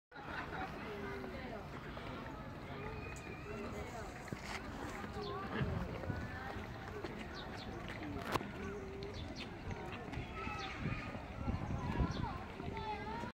20190501_서울대공원_라마
라마가 움직이는 모습을 보고 찍었습니다.